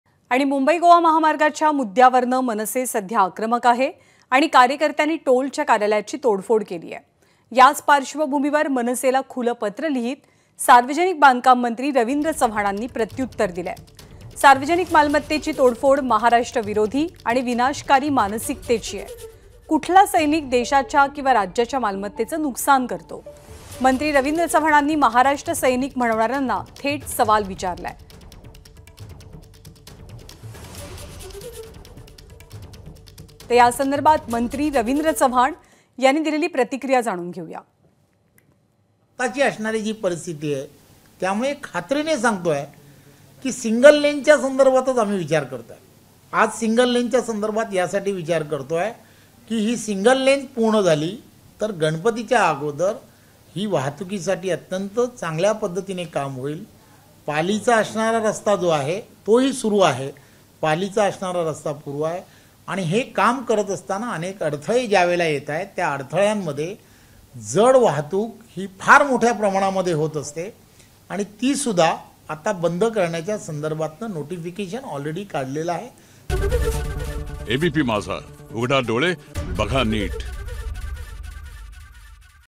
Ravindra Chavan: मंत्री रविंद्र चव्हाण यांचं खुल्या पत्रातून मनसेला खरमरीत उत्तर
Ravindra Chavan: मंत्री रविंद्र चव्हाण यांचं खुल्या पत्रातून मनसेला खरमरीत उत्तर मुंबई-गोवा महामार्गावरून मनसे आक्रमक झाली असून त्यांच्या कार्यकर्त्यांनी टोलच्या कार्यालयाची तोडफोड केलीय. या पार्श्वभूमीवर मनसेला खुलं पत्र लिहून सार्वजनिक बांधकाममंत्री रवींद्र चव्हाण यांनी जोरदार प्रत्युत्तर दिलंय